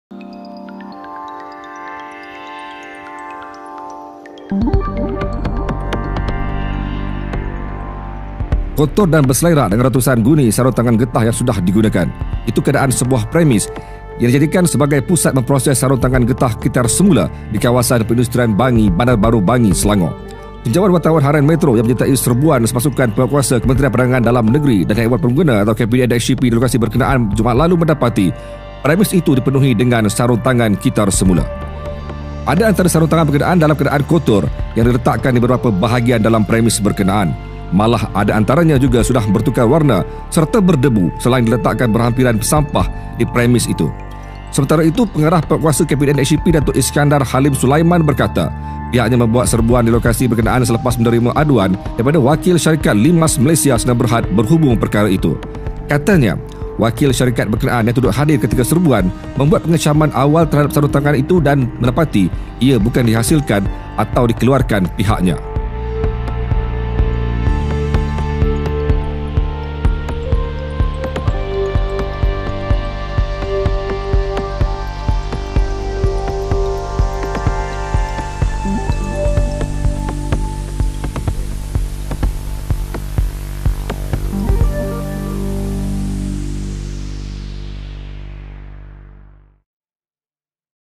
[1.12.2020] Sarung tangan dah guna dijual semula
Syarikat di Bangi, Selangor dikesan mengitar semula sarung tangan terpakai yang kotor dan menjijikkan sebelum dibersih dan dijual untuk pasaran tempatan serta antarabangsa.

Sarung tangan kitar semula itu pula diimport dari negara jiran yang dipercayai dikutip dari tempat pembuangan sampah klinikal.

Sumber: Harian Metro